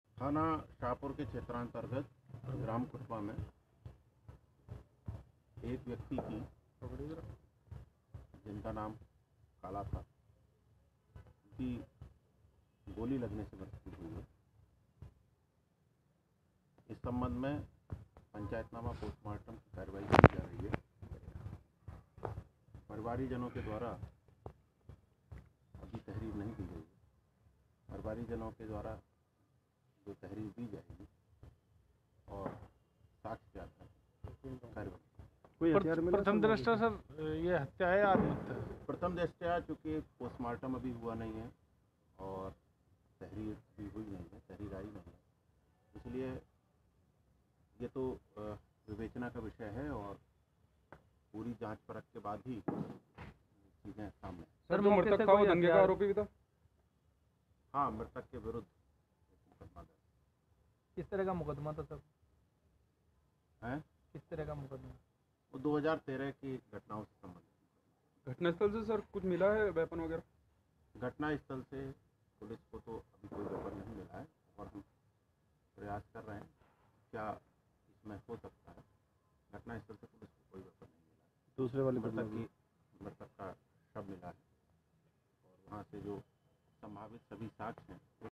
मुजफ्फरनगर दंगों में नामजद रहे आरोपी की हत्या हुई या आत्महत्या, पुलिस ही उलझी
मुजफ्फरनगर: 2013 में जनपद में हुए दंगों में नामजद रहे एक आरोपी काला की मौत की गुत्थी सुलझाना पुलिस के लिए टेढ़ी खीर साबित हो रही है. शनिवार को गोली लगने से युवक की मौत के मामले में चर्चाओं का बाजार गर्म है. पुलिस को यह समझ नहीं आ रहा है कि यह मामला हत्या का है या आत्महत्या का है.